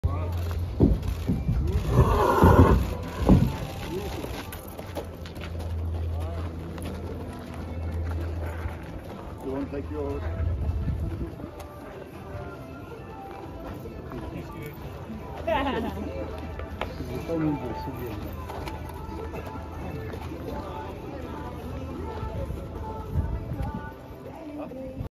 Etonnant anländer till Solvalla
Titelförsvararen av Elitloppet anlände under fredagen till Solvalla där han ska stå hela helgen, fram till avgörande på söndag. Etonnant vann Elitloppet 2022 och är här för att försvara titeln 2023.